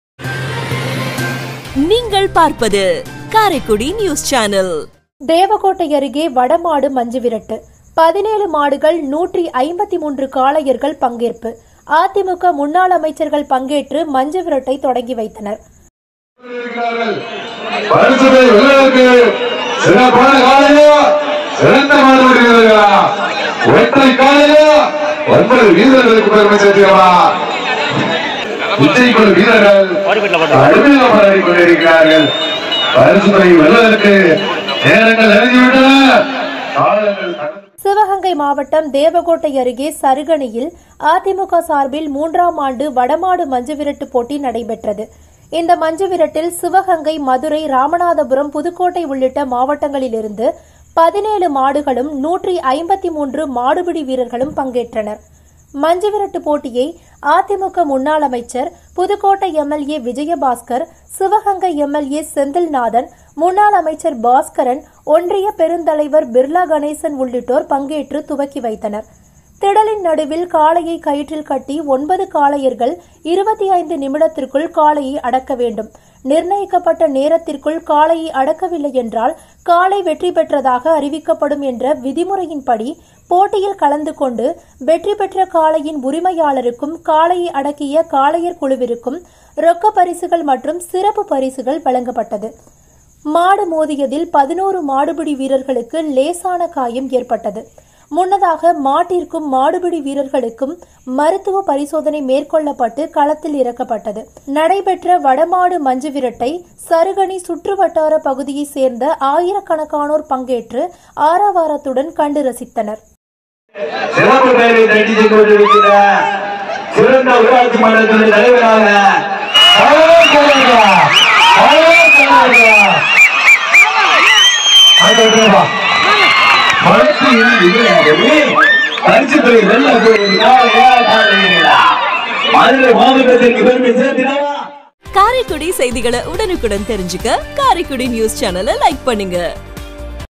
தேவகோட்டை அருகே வடமாடு மஞ்சுவிரட்டு. 17 மாடுகள் 153 காளையர்கள் பங்கேற்பு...
தேவகோட்டை அருகே வடமாடு மஞ்சுவிரட்டு. 17 மாடுகள் 153 காளையர்கள் பங்கேற்பு. அதிமுக முன்னாள் அமைச்சர்கள் பங்கேற்று மஞ்சுவிரட்டை தொடங்கி வைத்தனர்.